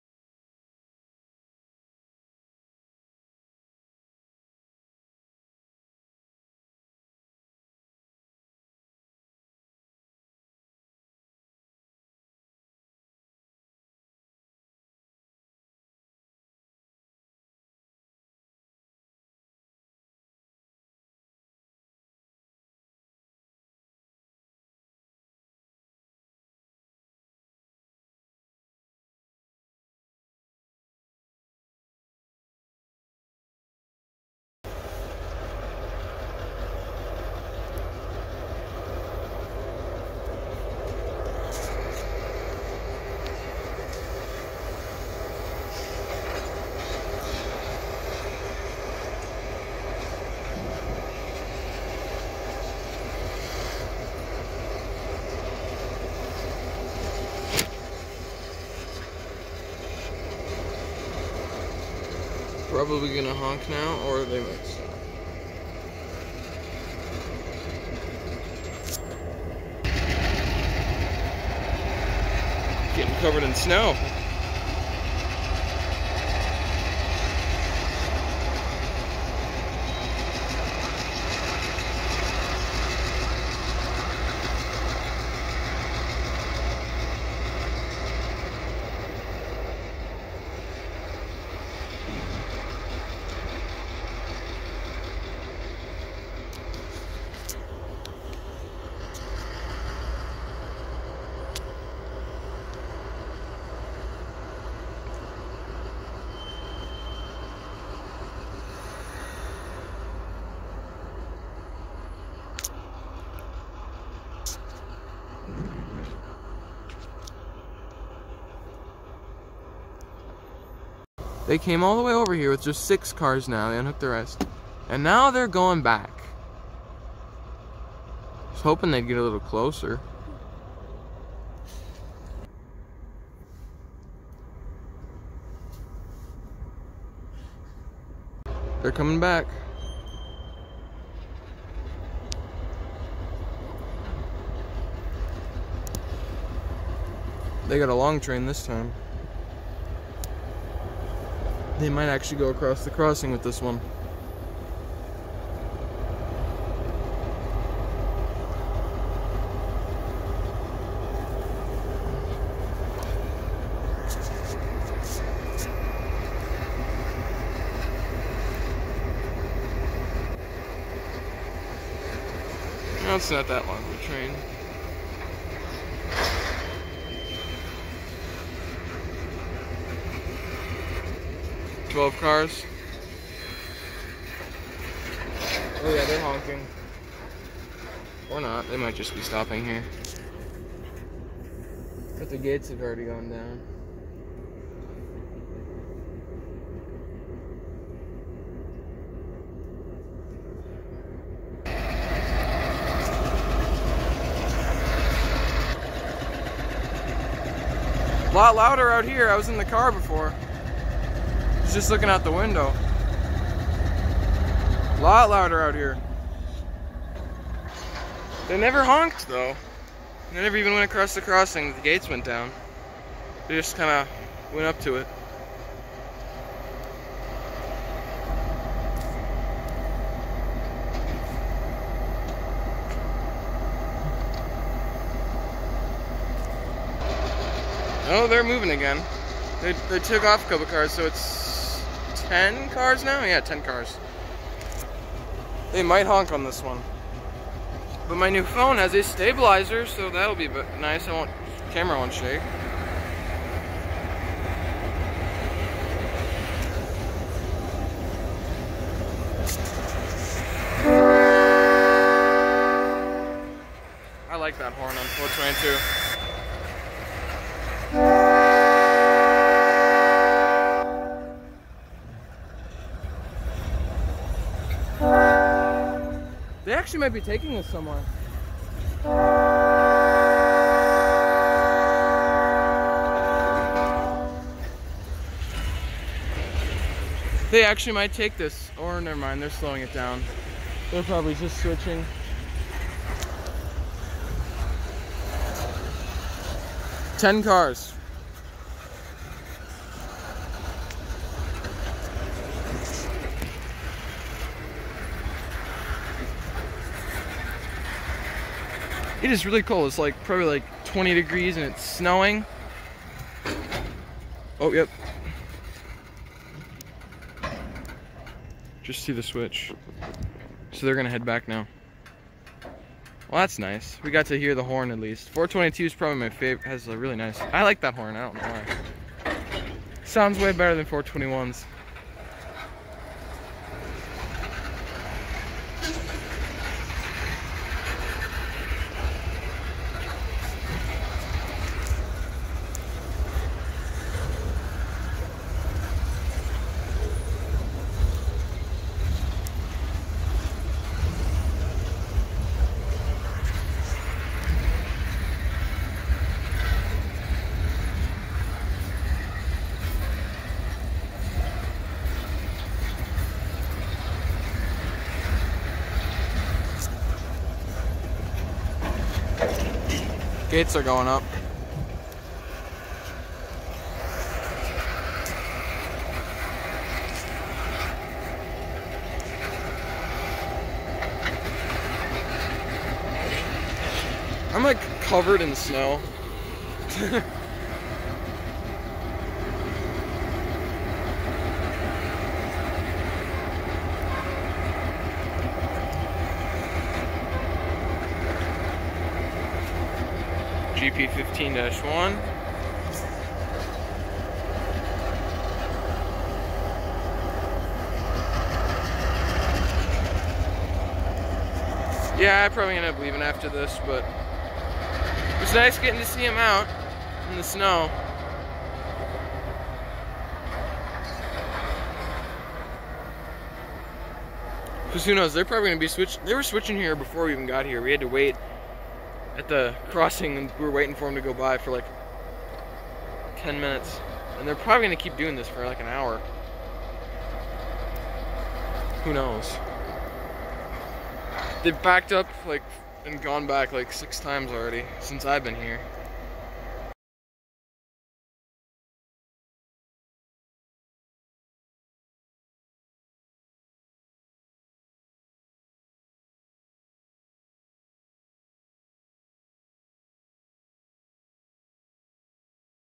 Northern Lines Railway Switching In The Snow
Sorry for the late upload, there isn't much time for editing during the holidays and it's hard to get videos in the winter.